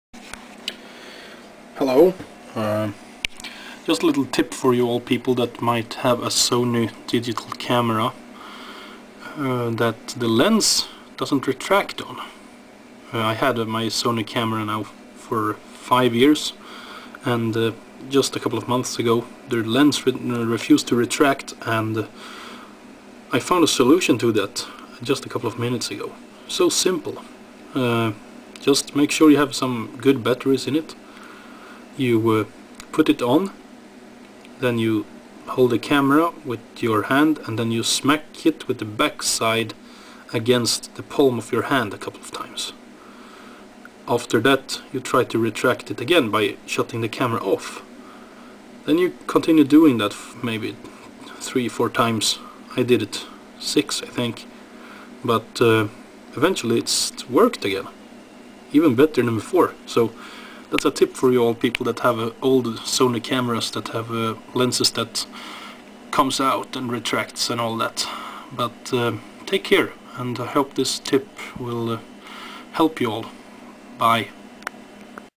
A tip on how to fix your Sony camera Part 1
Incase the lens fails to retract with your Sony camera I have a tip.
1. Put it on
2. Smack it with its backside to your palm a couple of times
3. Put it off
Do the procedure a couple of times and hopefully the lens will retract again and you dont need to buy a new camera or have a expensive repair!
If you need any examples on how to do I have another video up that shows how to do it
Good luck!